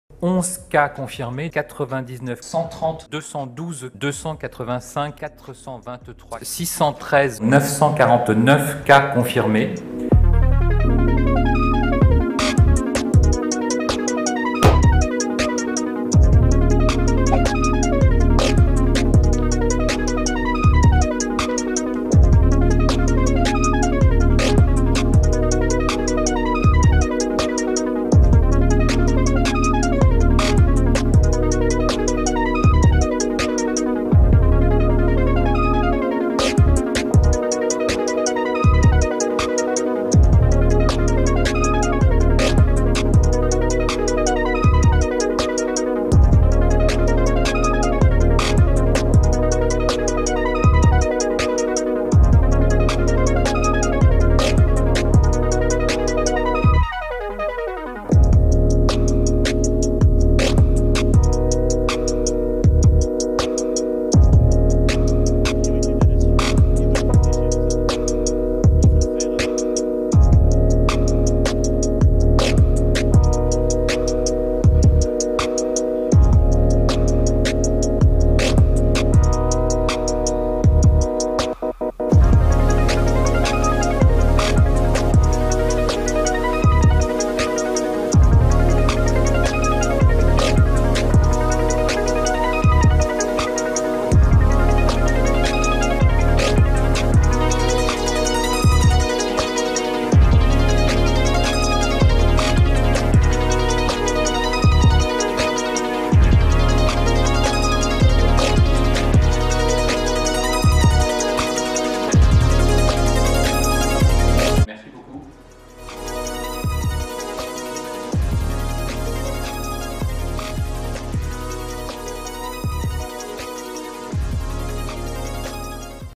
Coronavirus : qui est Jérôme Salomon ?
Chaque jour, du haut de son pupitre, Jérôme Salomon s’adresse à la presse pour faire le décompte des malades du coronavirus. Médecin infectiologue, professeur des Universités, chef de clinique, haut fonctionnaire de l’Etat, mais aussi amateur de musique et homme discret. Qui est Jérôme Salomon, l’homme passionné par les virus ?